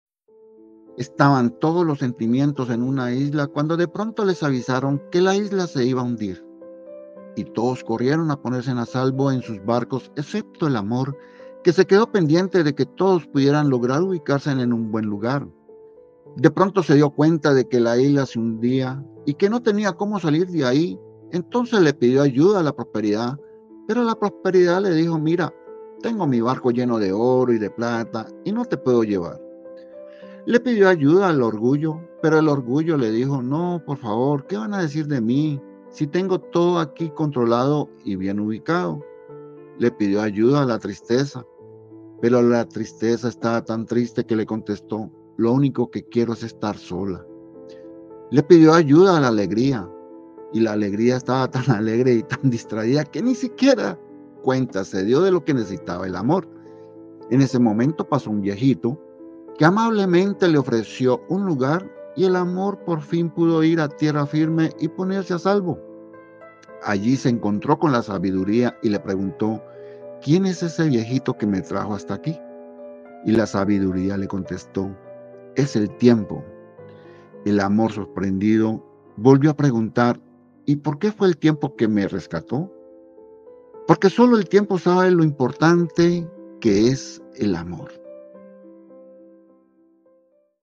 La Isla de los Sentimientos. / Reflexión
Un cuento de Jorge Bucay, una interesante metáfora sobre las diferentes emociones que podemos experimentar.
"Creo que el mundo no es un lugar para competir, sino para compartir, y una condición necesaria es mirarse a uno mismo"
Jorge Bucay.
Audio: Enrique Salinas
Foto: Isla de Buracay II - Filipinas.